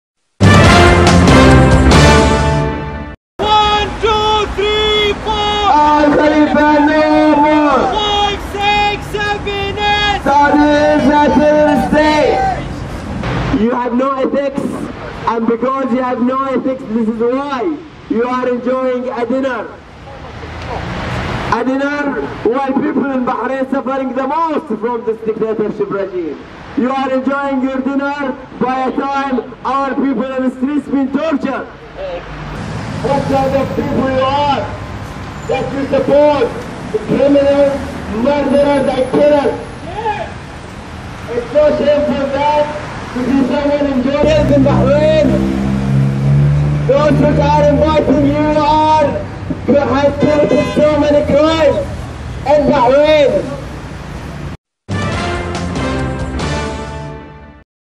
المعارضة تعتصم أمام فندق الدوشستر في لندن فيما يحتفل أزلام النظام في الداخل 9/12/2013
الشعارات تدوي بالقرب من فندق الدوشستر في حين تناول الخليفين وضيوفهم العشاء واحتساء الكؤوس على دماء الشعب